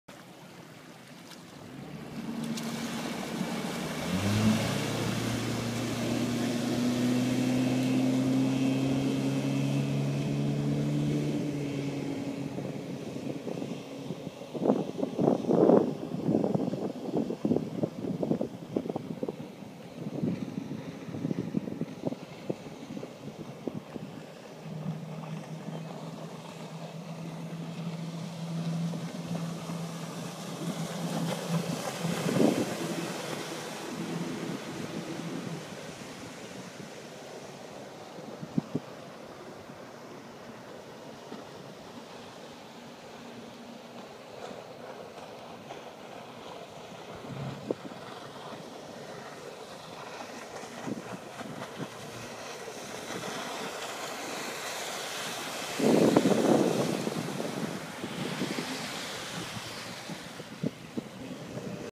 2012 17' Bayliner - 170OB
90HP Mercury four stroke.
Very quiet and efficient. No smoke.
for more information!